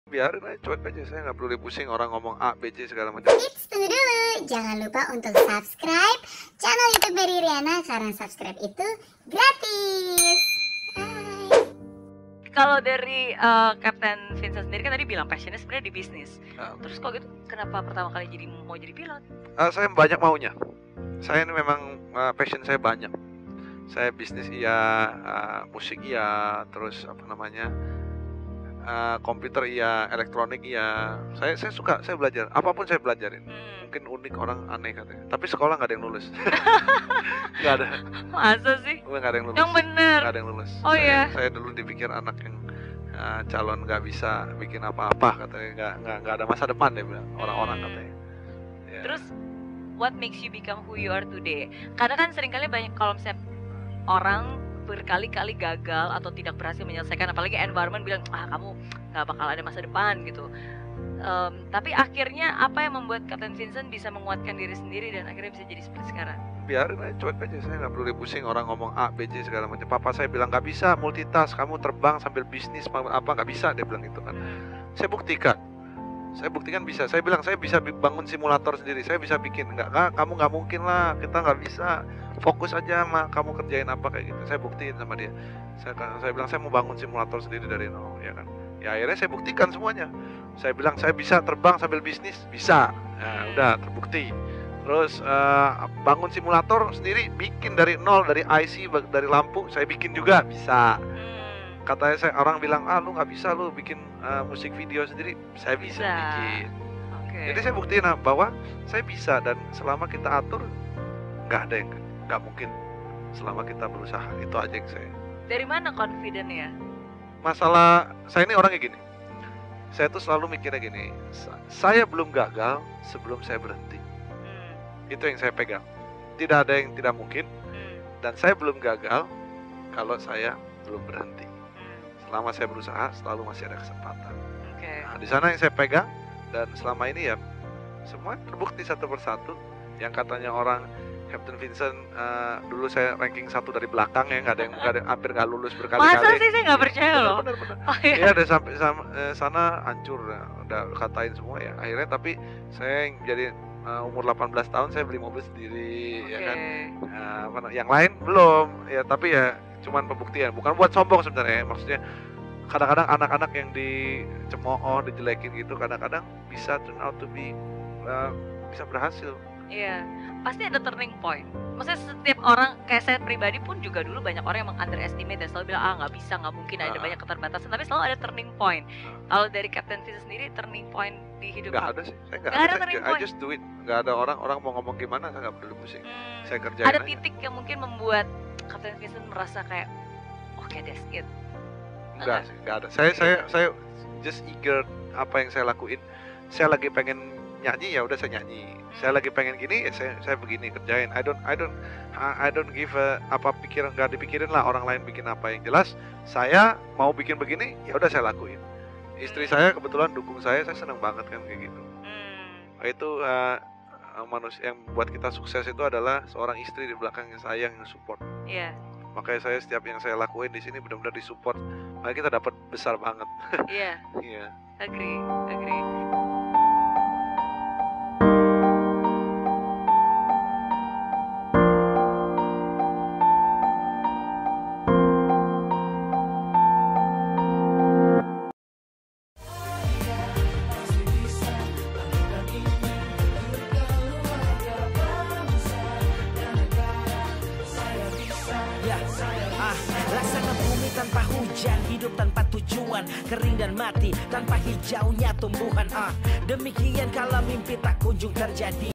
SEBELUM LICENSI PILOTNYA DICABUT, CAPT. VINCENT RADITYA CURHAT INI KE MERRY RIANA | Cool Collab
Dibalik problematika tentang Lisensi terbang single engine milik Kapten Vincent Raditya, dicabut oleh Kementerian Perhubungan sebagai regulatornya. Pencabutan lisensi single engine Kapten Vincent pun menjadi perbincangan akhir-ahkir ini. siapa sangka dibalik semua itu ada cerita haru yang sebelumnya pernah disharing kan ke saya...

Thank you Kapten Vincent untuk collabnya, Nah pastikan kamu juga nantikan video keseruan saya yang ada di channelnya Capt.